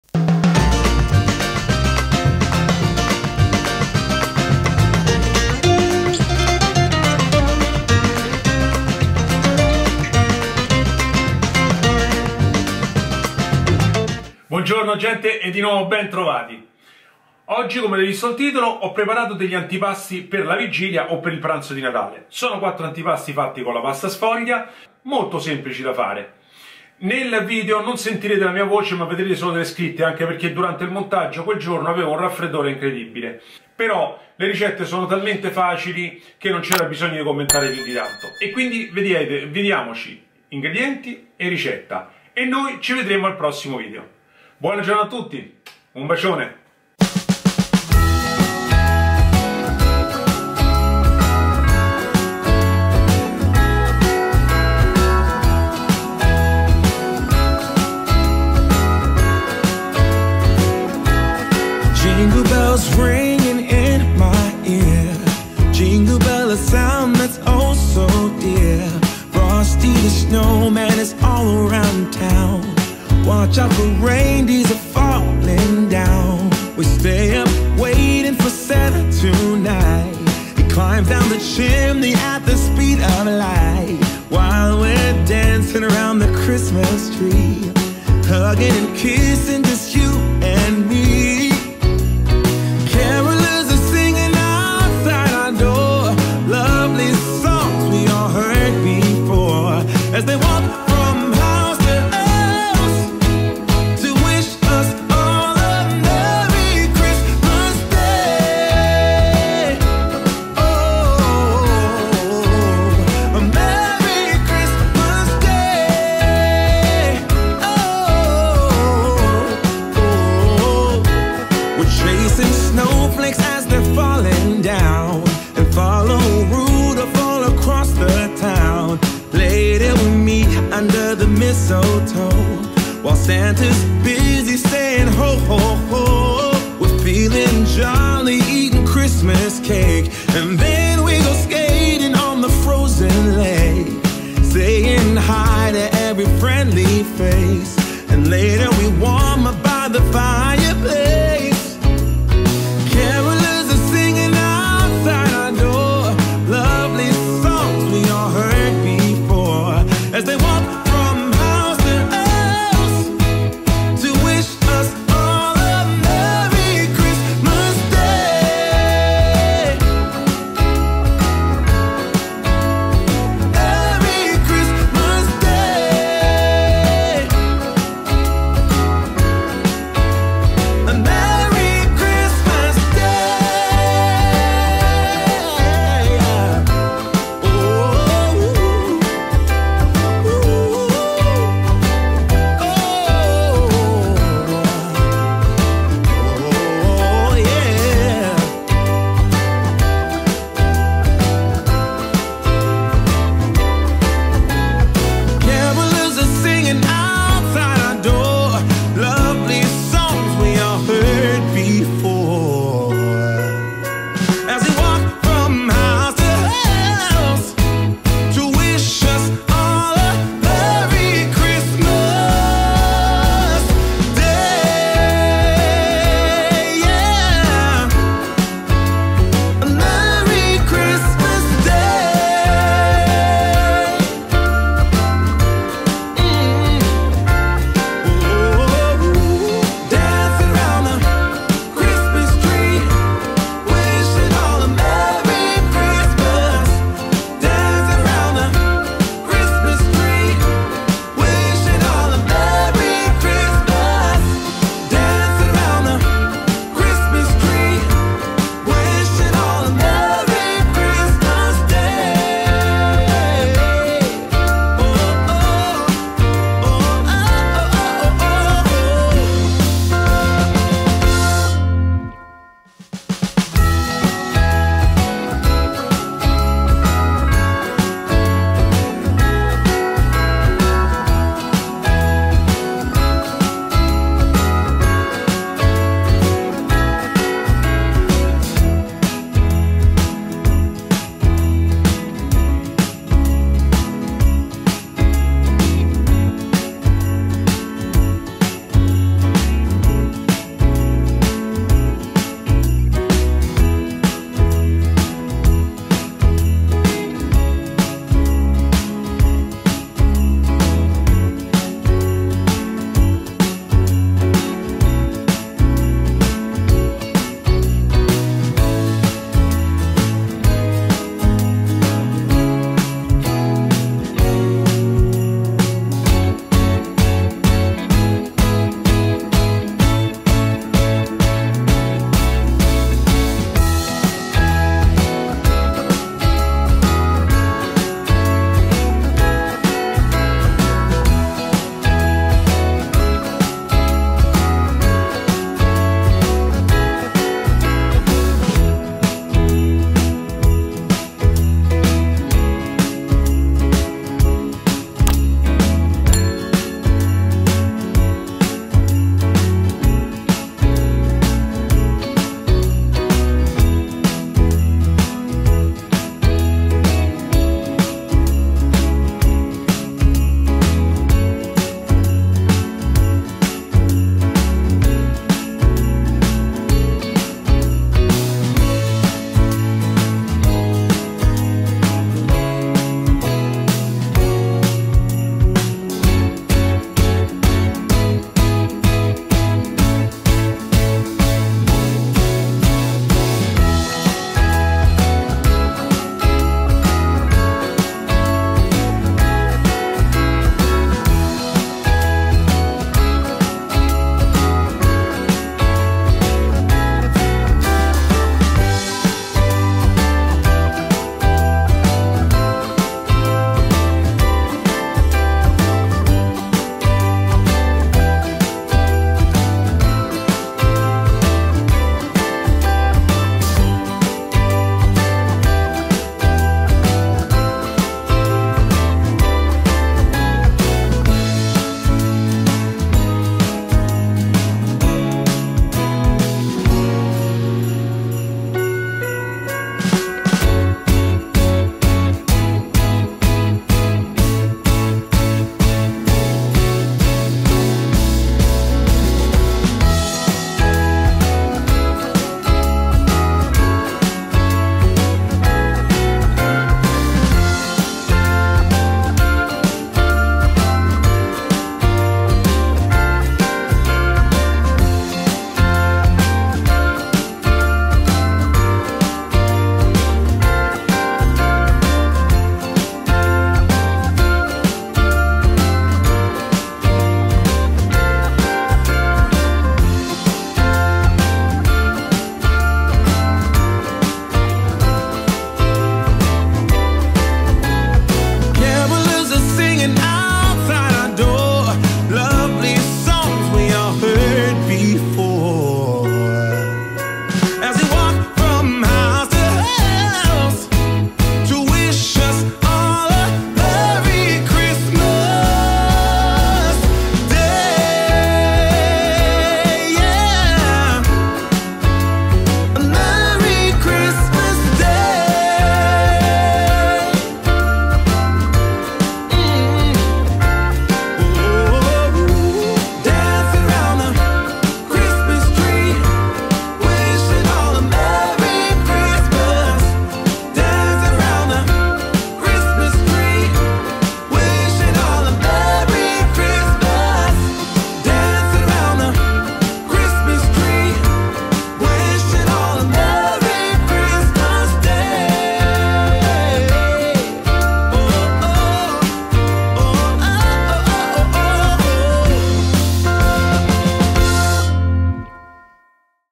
4 Antipasti di Natale di pasta sfoglia VELOCI E SFIZIOSI (SUB ENG-FRA-ESP-GER)
4 Antipasti di Natale di pasta sfoglia VELOCI E SFIZIOSI (SUB ENG-FRA-ESP-GER)

Ecco per voi 4 ricette facili e veloci per impreziosire la vostra tavola nel periodo natalizio, 4 ricette con la pasta sfoglia che delizieranno i vostri cari nel periodo più atteso.
Questi gli ingredienti delle 4 ricette

Tartellette al formaggio e cipolla rossa:
1 rotolo di pasta sfoglia rettangolare
150 gr di formaggio spalmabile
1 cipolla rossa
Sale

Stelle di sfoglia con prosciutto e formaggio:
1 rotolo di pasta sfoglia rettangolare
100 gr di prosciutto cotto in 1 fetta sola
100 gr di scamorza dolce
Semi per decorare (sesamo bianco e nero o papavero)

Vol au vent con formaggio alle erbe:
1 rotolo di pasta sfoglia rettangolare
250 gr formaggio spalmabile
Erbe aromatiche (erba cipollina, prezzemolo, basilico)
1 uovo

Pizzette rustiche:
1 rotolo pasta sfoglia rettangolare
100 gr passato dipomodoro
Sale
origano essiccato

00:00 Introduzione
00:55 Tartellette formaggio e cipolla
02:44 Stelle di sfoglia al formaggio e prosciutto
04:54 Vol au Vent con crema al formaggio
08:05 Pizzette rustiche